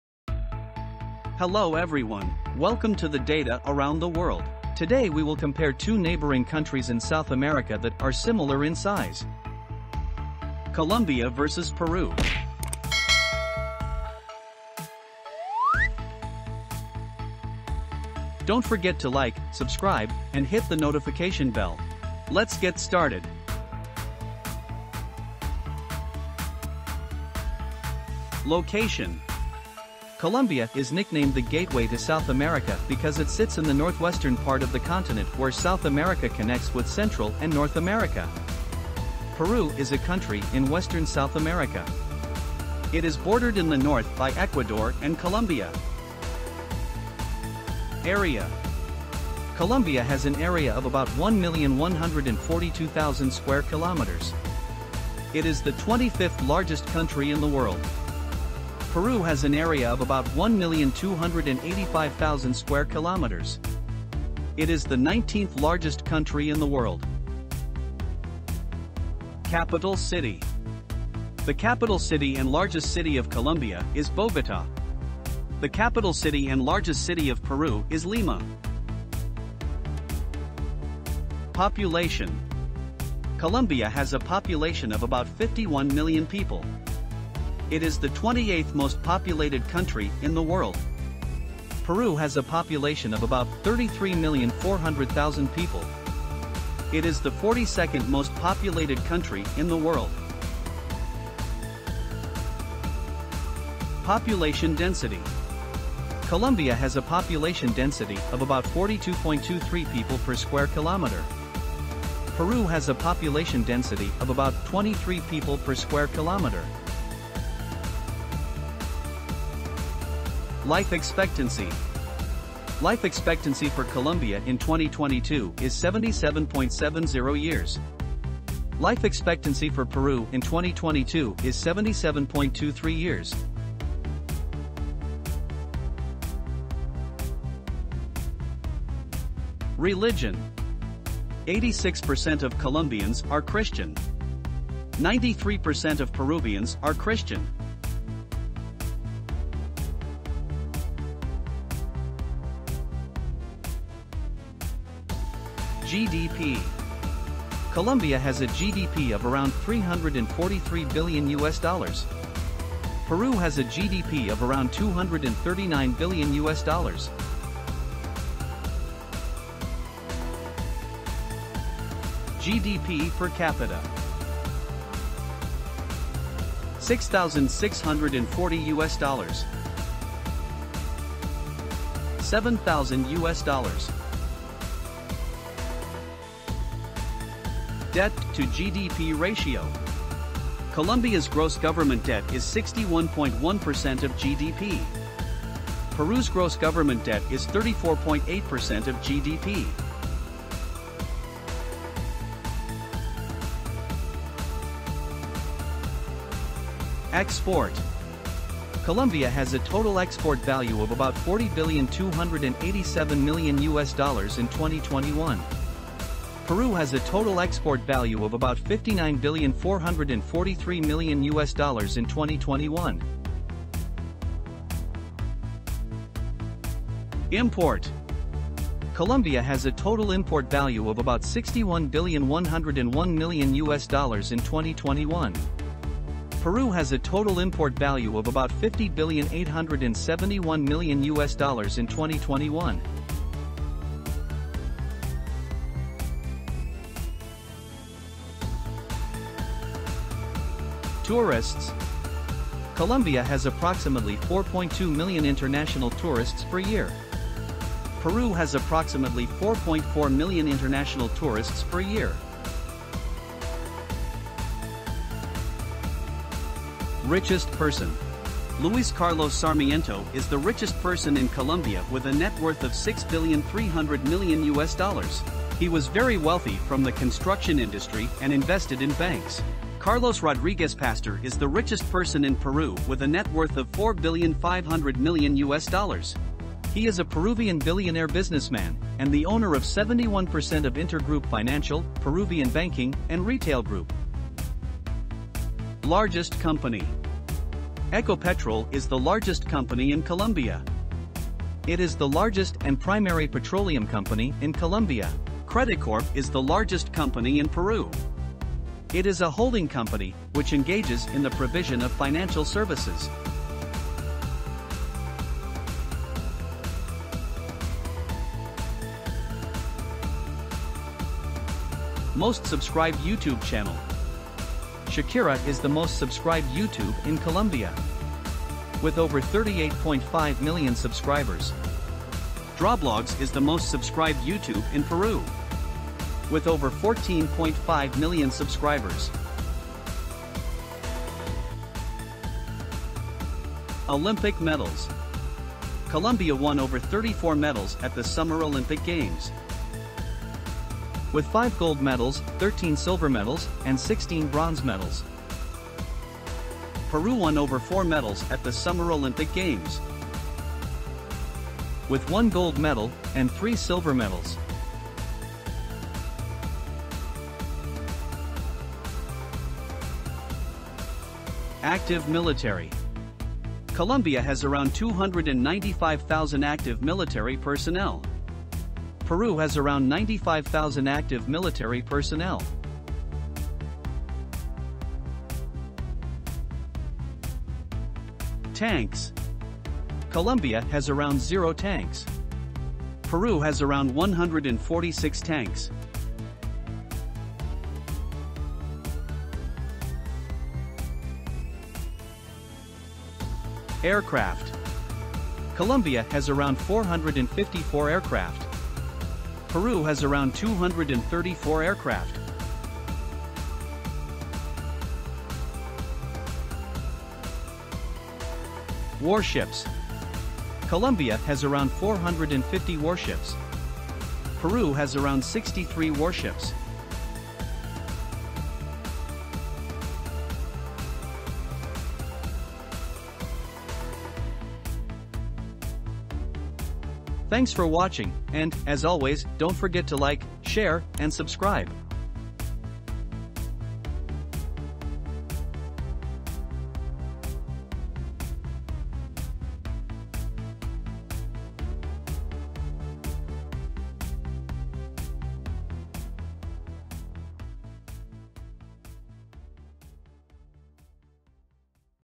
Colombia vs Peru - Country Comparison 2022
Colombia is nicknamed the "gateway to South America" because it sits in the northwestern part of the continent where South America connects with Central and North America.
Peru is a country in western South America. It is bordered in the north by Ecuador and Colombia .

Source
tradingeconomics
wikipedia
IMF
WTO
UNWTO
forbes
socialblade
globalfirepower